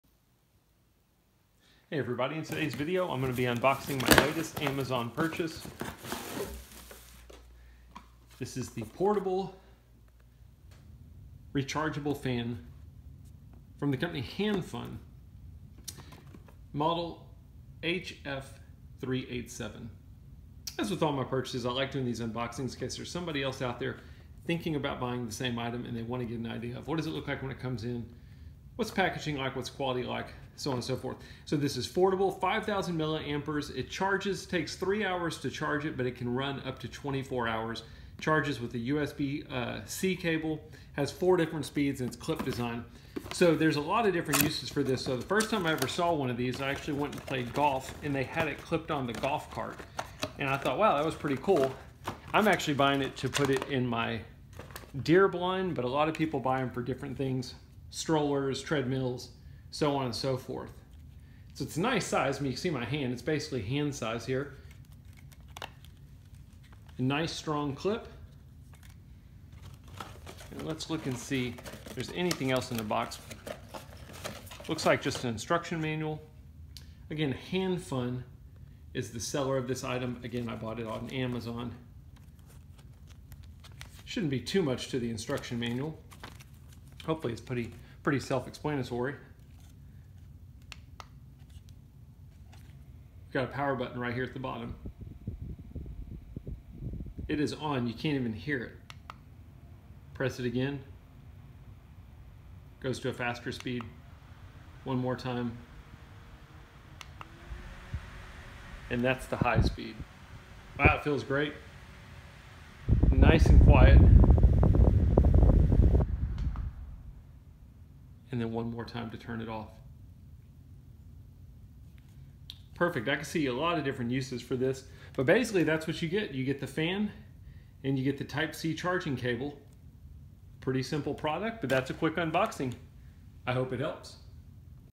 Portable / Rechargeable Fan (for Golf Carts, Strollers, etc.) by Hand Fun - Unboxing
See Amazon listing here for more information on this item
This is an un-boxing video of the rechargeable, portable, oscillating fan by the seller Hand Fun on Amazon.  Item is perfect for the baby stroller, golf cart, deer stand, etc.